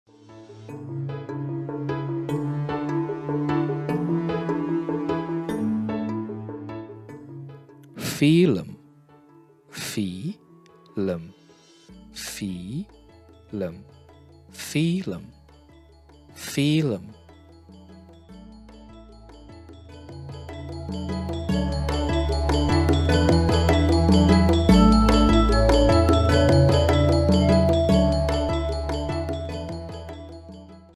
Let's Speak Bahasa Indonesia - Film
Today we’ll give you one of the easiest BahasaIndonesia word for you to remember, which is film! Yes, it is exactly the same spelling as the English translation!

DidYouKnow that every March 30th, we commemorate Indonesian National Film Day? Share in the comment if you have a favorite Indonesian film!